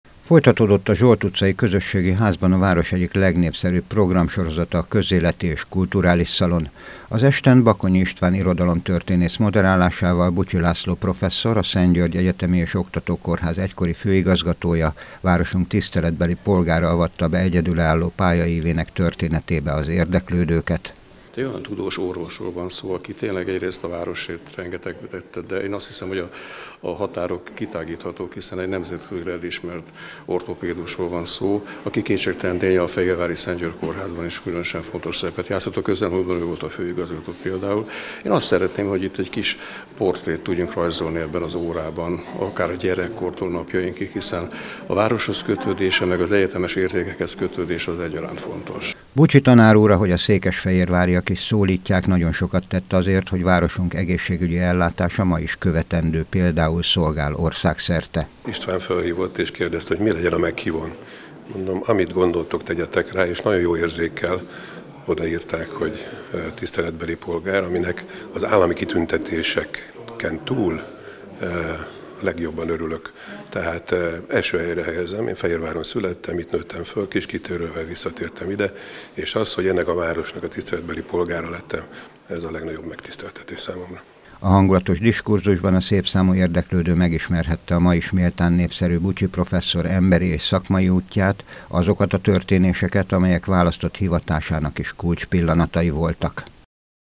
KÖZÉLETI ÉS KULTURÁLIS SZALON
Bucsi László professzor a Szent György Kórház volt főigazgatója volt a Közéleti és Kulturális Szalon vendége. Városunk tiszteletbeli polgárával Bakonyi István beszélgetett életútjáról.